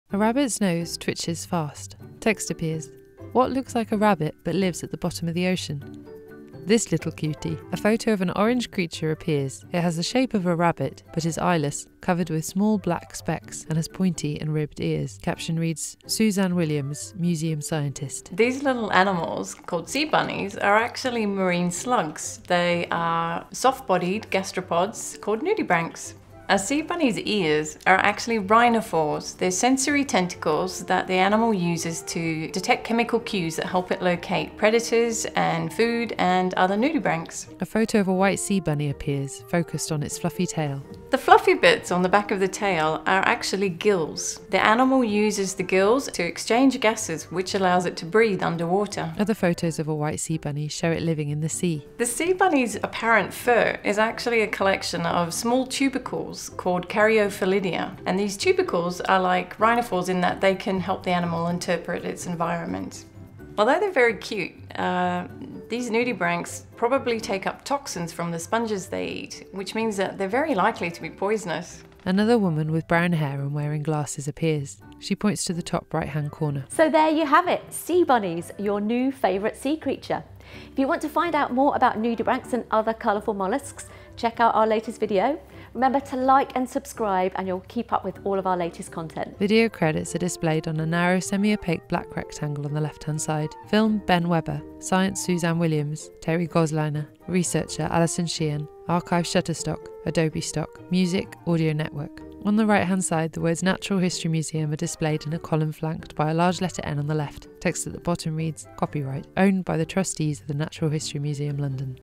What is a sea bunny? (Audio Described)
Have you ever heard of a sea bunny?
These adorable, soft-bodied creatures are part of a group of animals called nudibranchs.
While these sea slugs bear a resemblance to a rabbit, their features are actually functional. Museum Scientist Suzanne Williams explains.

Find out more about nudibranchs and their diet of toxic creatures